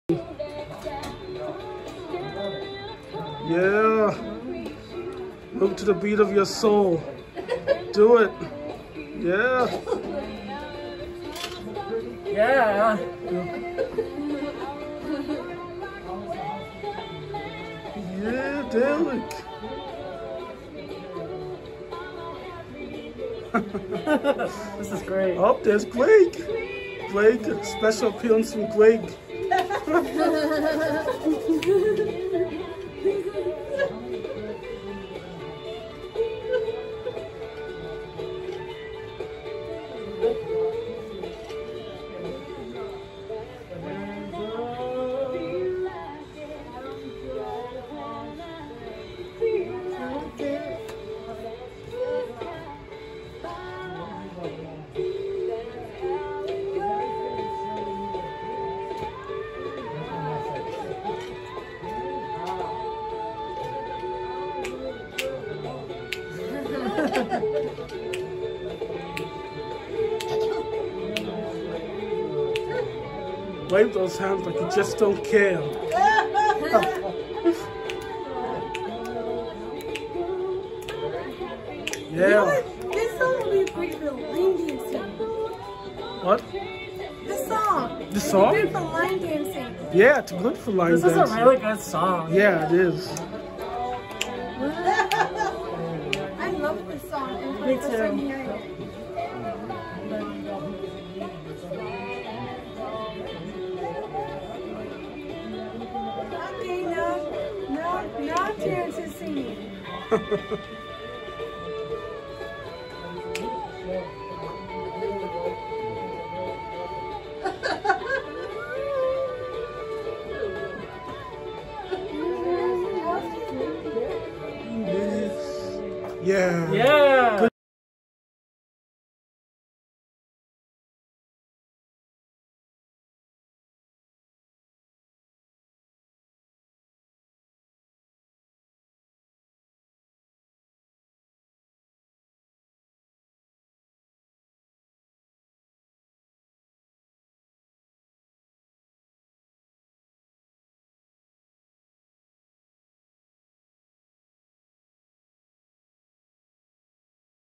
Derek Frazier Dancing at NCA/Fish Swimming Cartoon Animation
Heptagon Arts Studios & Aeather Inc. Presents Derek Frazier Cartoon Animation
Video By Yinneboma Peterson
Filmed By Yinneboma Peterson
Cartoon & Animation By Animator/Cartoonist Yinneboma Peterson.